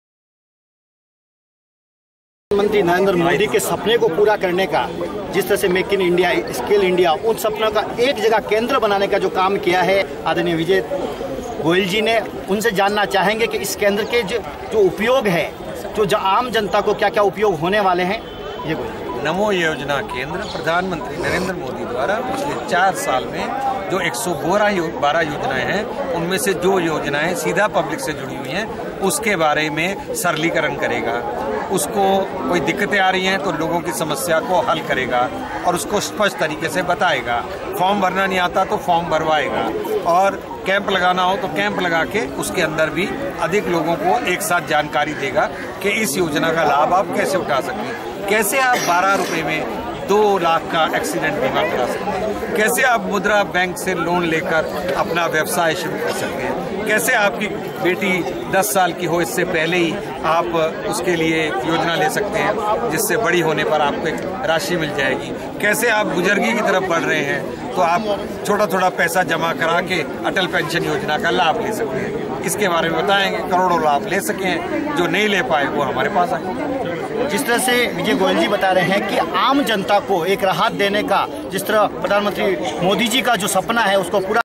NEWS MAKE IN INDIA
नमो योजना केंद्र-न्यूज़ मेक इन इंडिया के साथ बातचीत करते हुए केंद्रीय मंत्री विजय गोयल